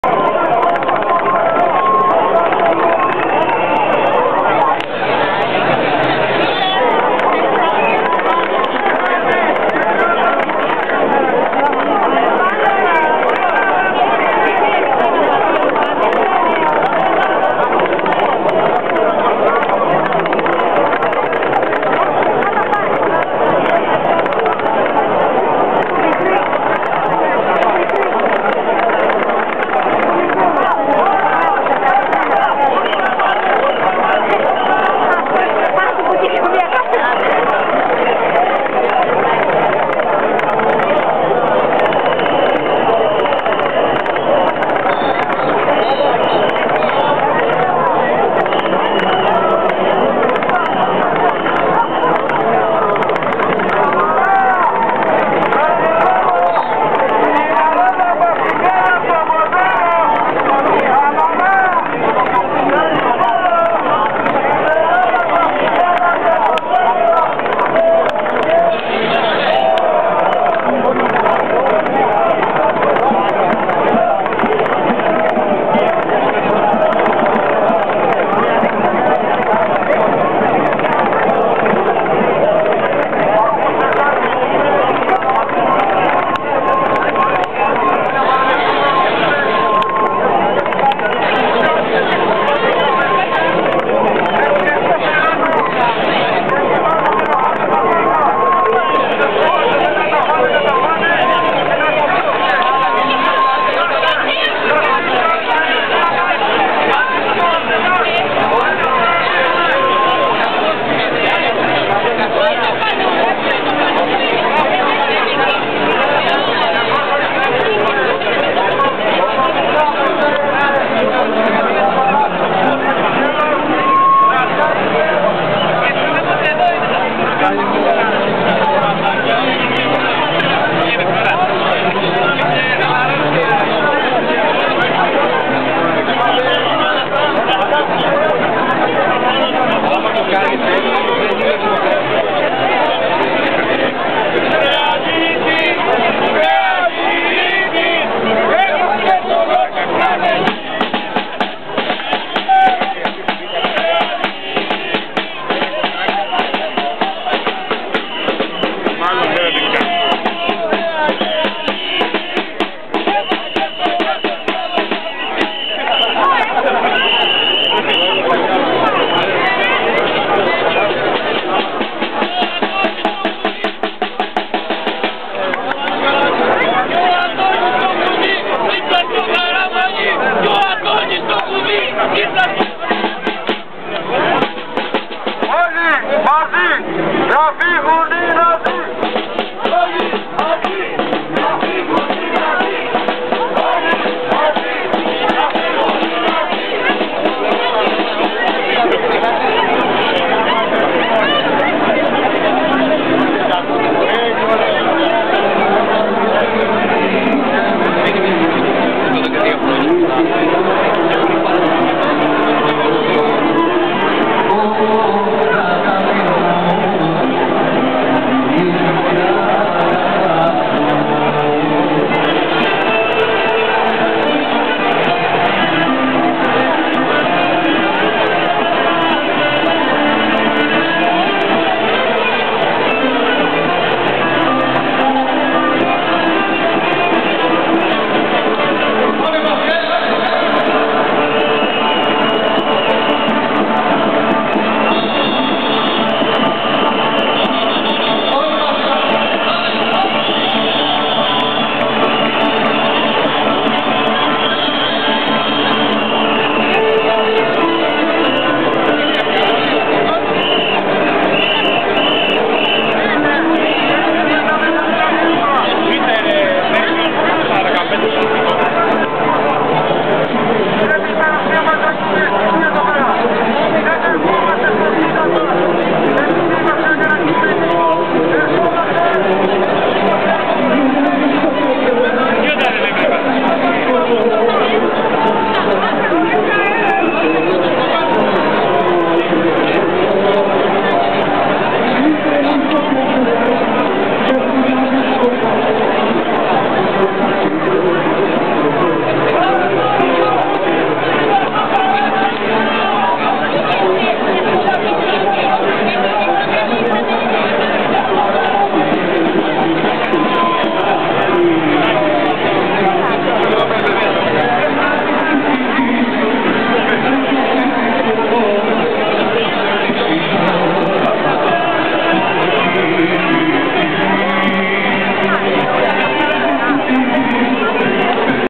16 Ιουλιου 2013 Νυχτερινο Συλλαλητηριο Βουλη Night demonstration greek Parliament. part 2
16 Ιουλιου 2013 Νυχτερινο Συλλαλητηριο Βουλη
16th July Night demonstration outside greek Parliament.